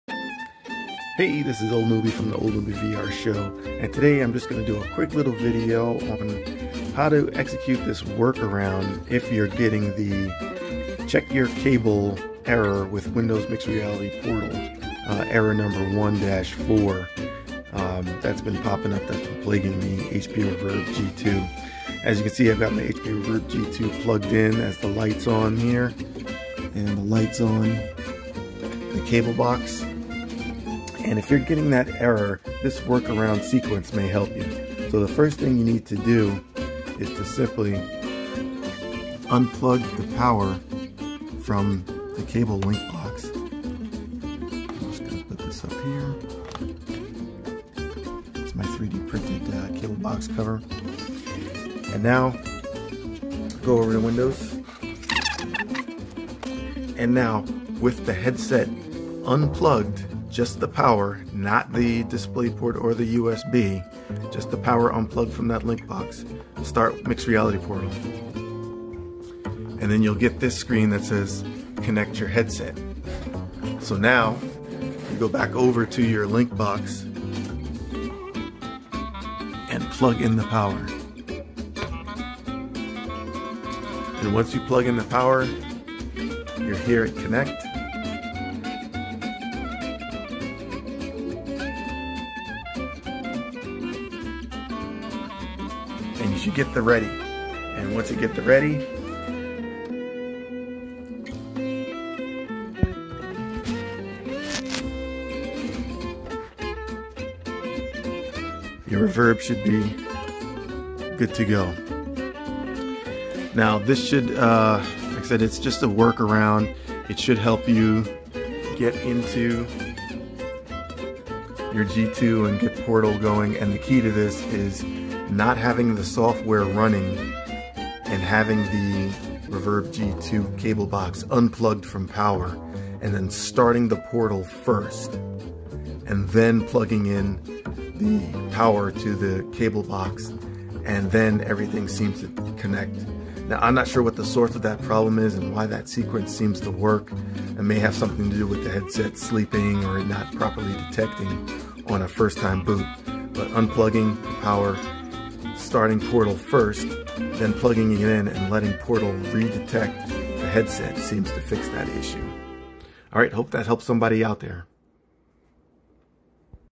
WMR Portal Error 1-4 Workaround
I get the 1-4 Error with my HP Reverb G2 (rev 1) regularly on boot, but this little workaround sequence gets me going past the error every time! Hope it helps!